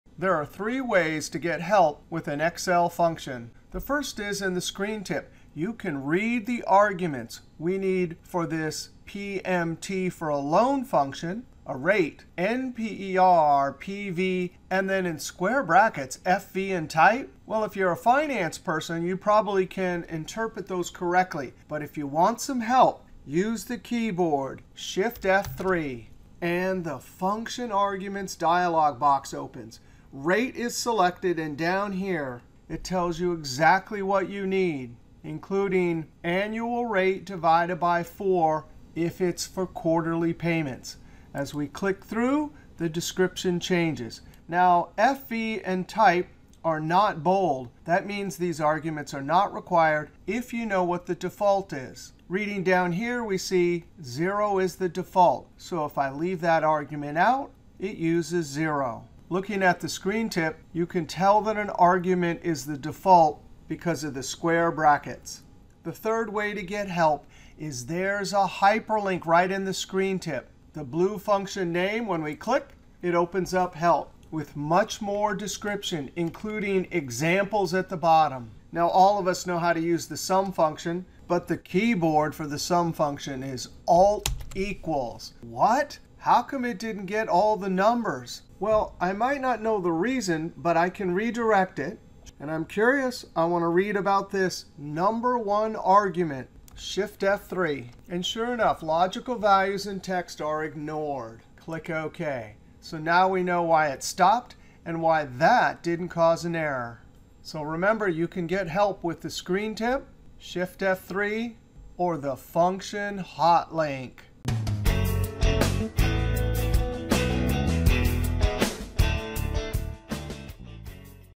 Instant Help with Excel Function: Hot Link, Keyboard or Screen Tip? Excel Magic Trick #1715
Learn three easy ways to get help instant help with Excel function arguments: 1) Hot Link to Help, 2) Keyboard to open Functions Argument dialog box (Shift + F3) or 3) Screen Tip.